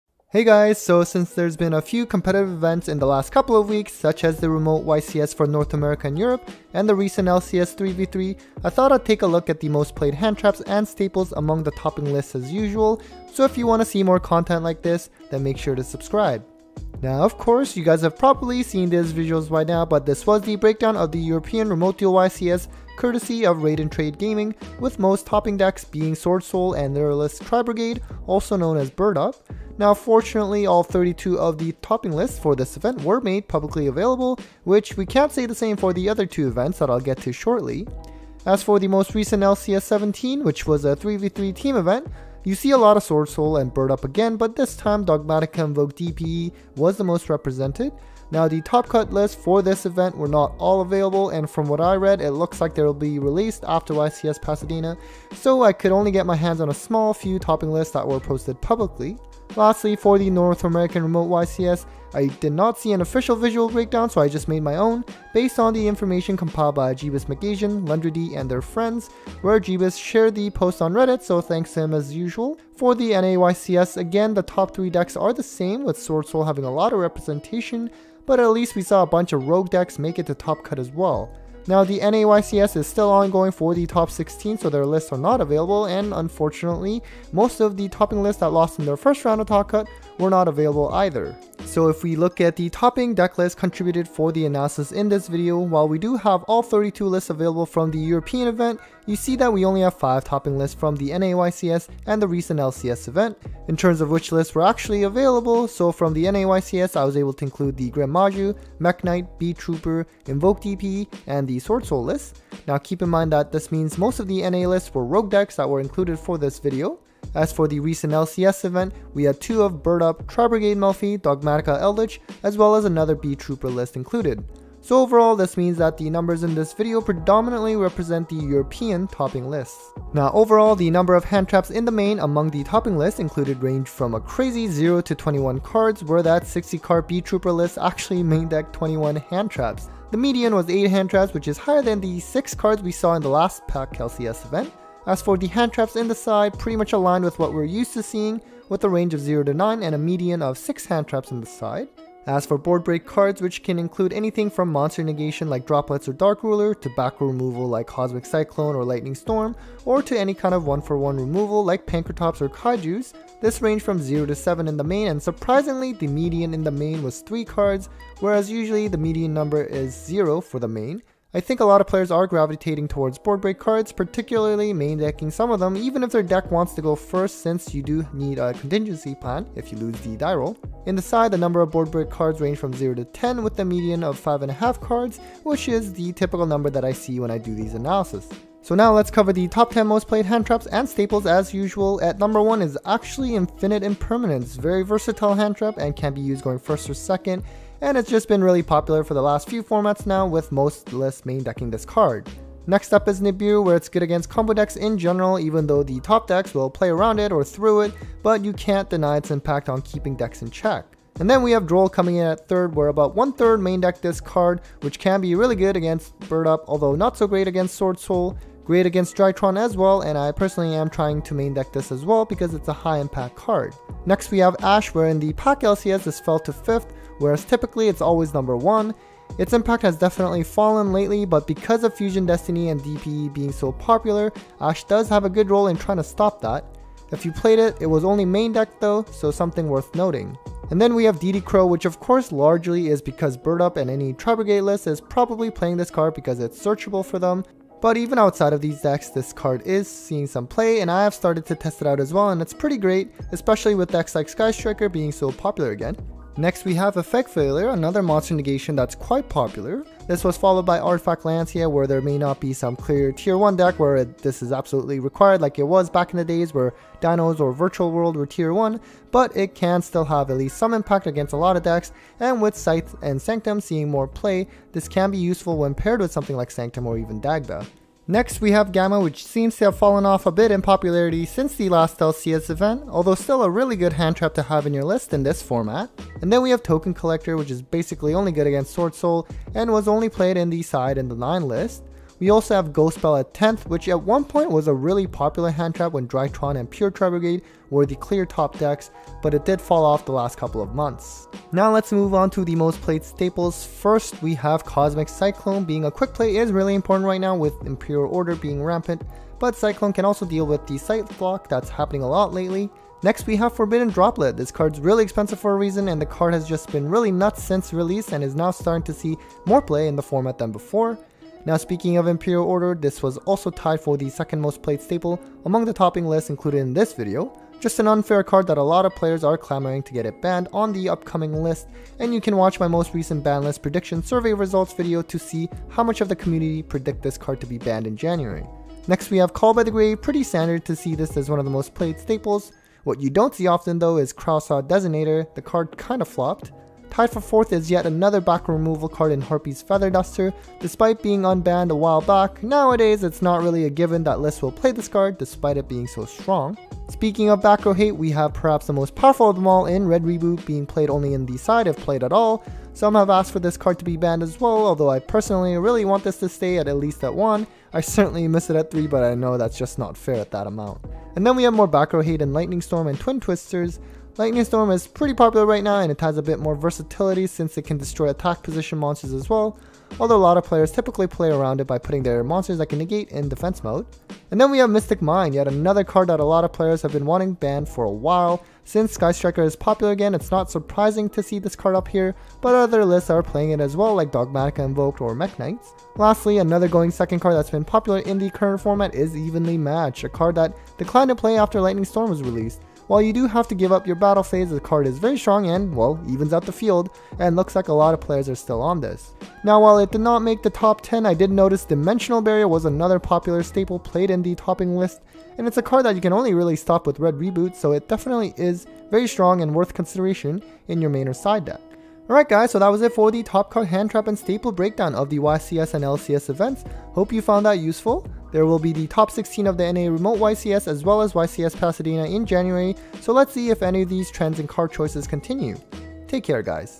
Yu-Gi-Oh! | Remote YCS and LCS Analytical Breakdown! Most Played Hand Traps and Staples | Dec 2021
Here is a breakdown of the most played hand traps and staples among some of the topping lists from the Remote YCS for Europe and North America, as well as the recent LCS 16 3v3 event! Most of the lists from the NA YCS and LCS were not publicly available, so I had to go with what I could find.
Which hand traps and staples do you think are necessary to combat the current meta of Swordsoul, Bird up, Invoked DPE, and more?